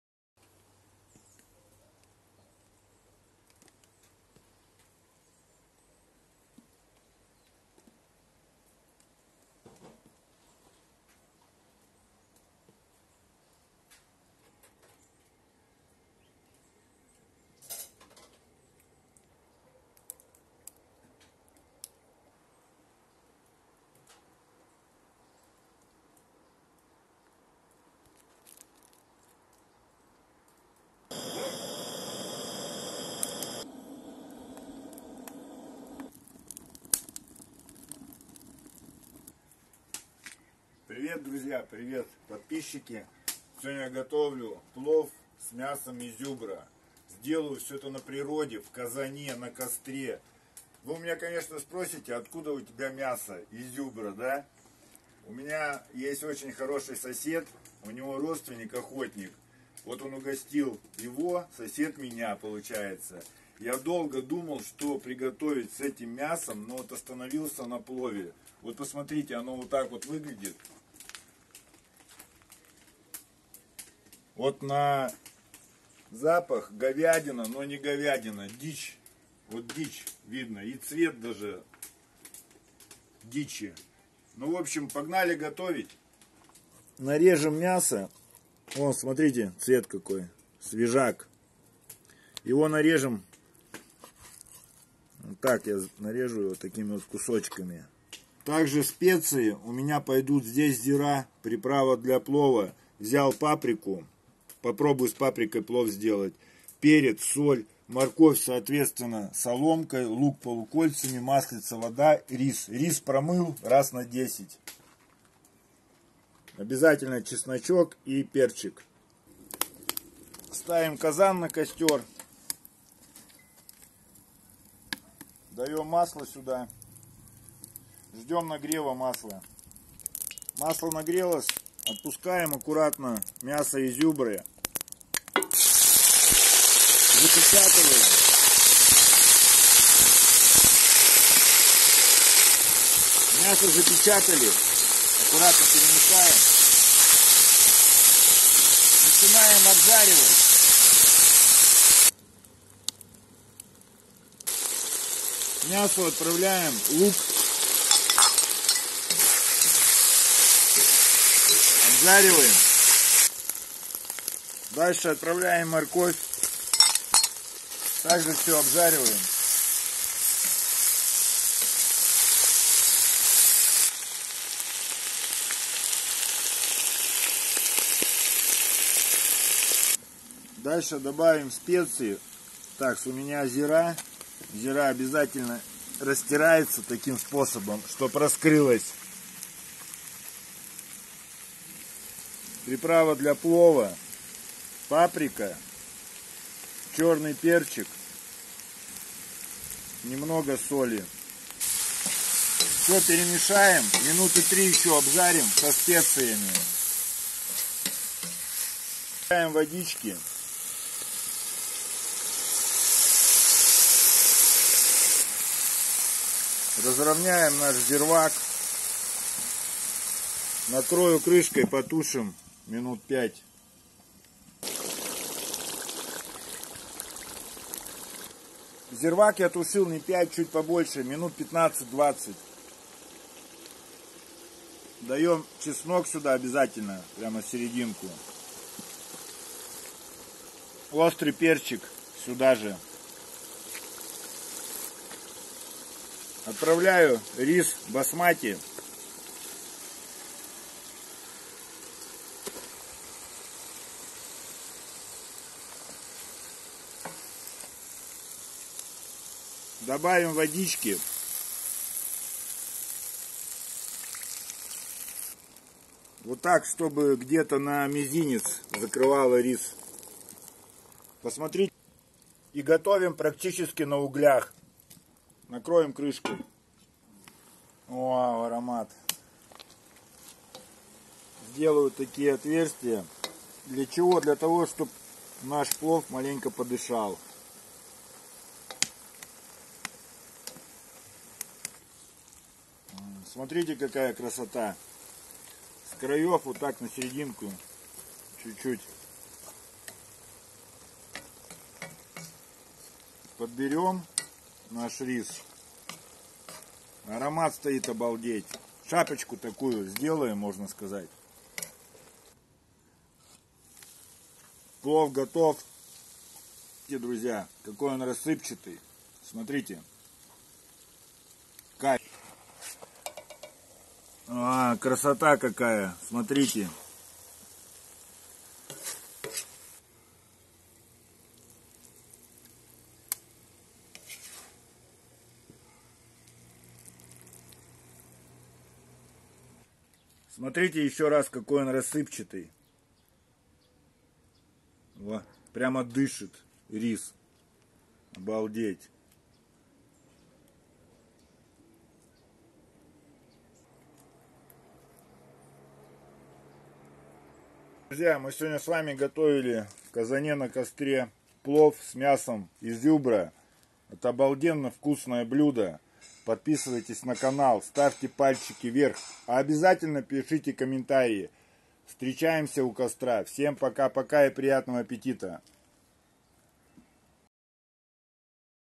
Настоящий ПЛОВ с мясом ИЗЮБРЯ в казане на костре
мясо зюбрятины чем то напоминает говядину,но на  запах и на вкус оно совершенно другое! плов удался,рис рассыпчатый,мясо зашло на УРА!